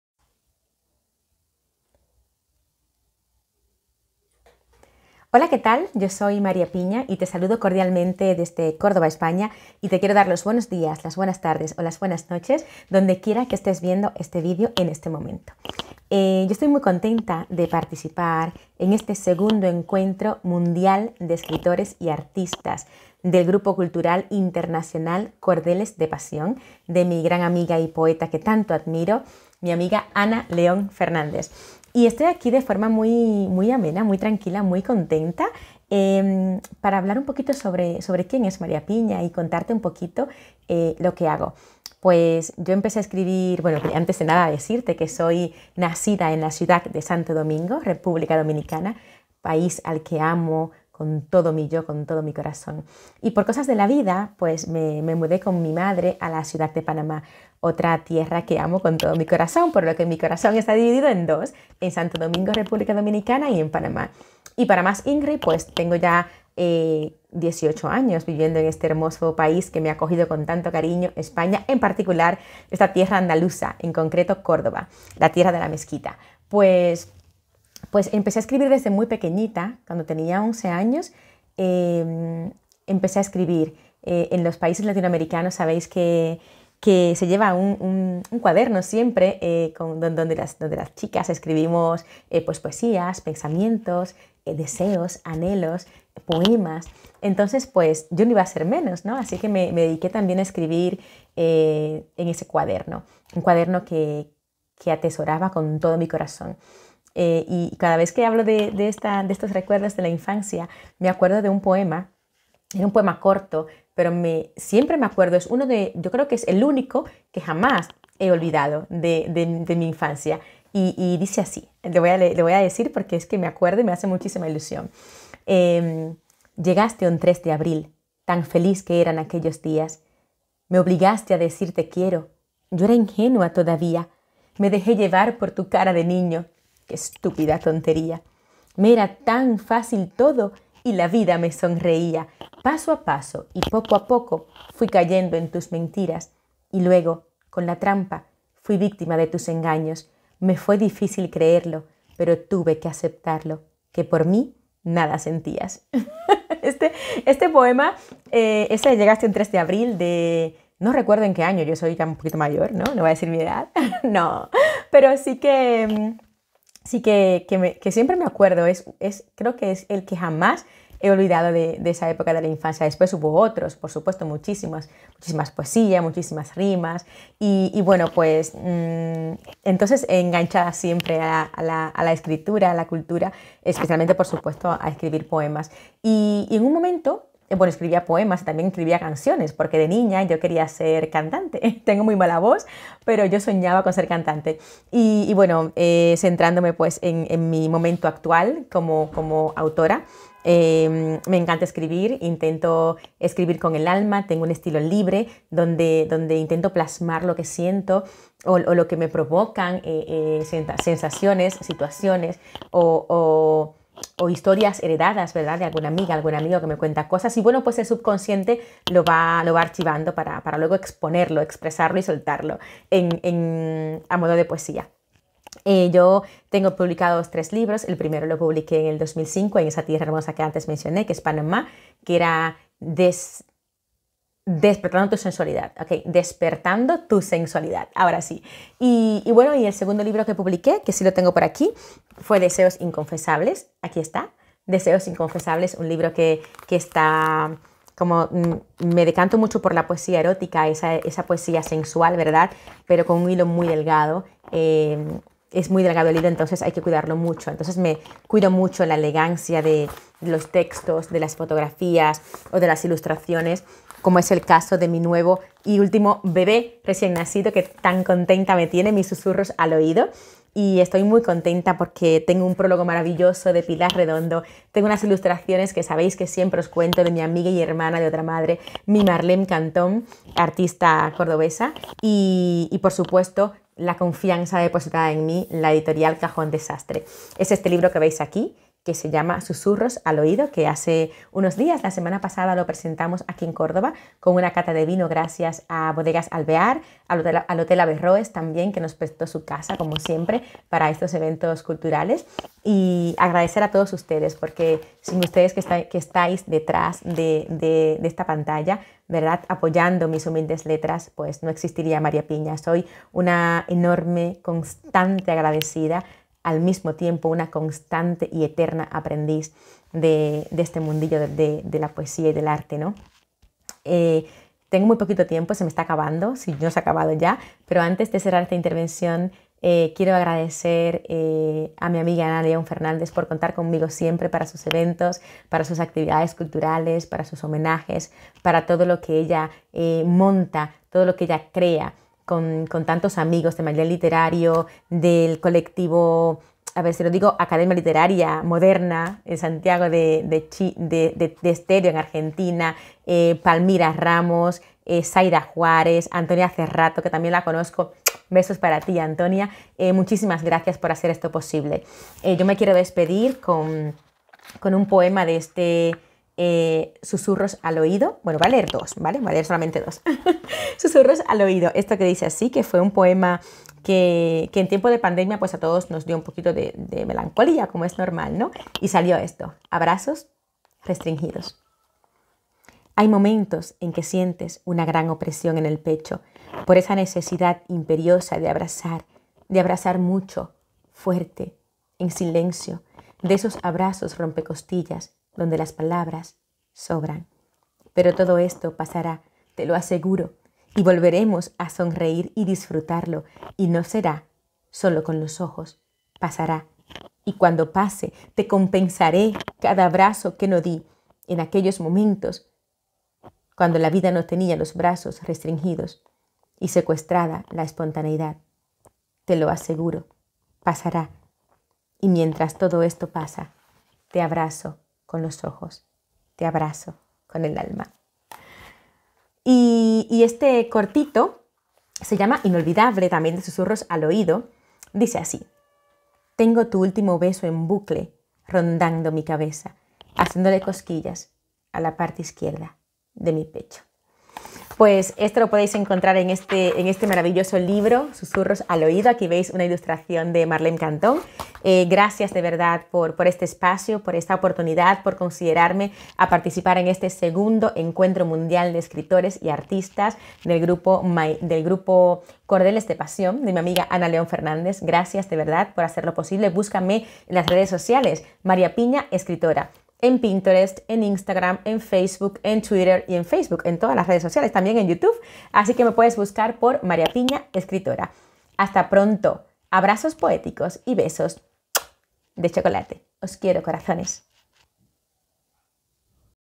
II Encuentro Mundial de Escritores y Artistas
Os comparto mi participación en este II Encuentro Mundial de Escritores y Artistas organizado por el Grupo Cultural Internacional Cordeles de Pasión, España. Gracias a mi querida  amiga Ana León Fdz. por  contar conmigo.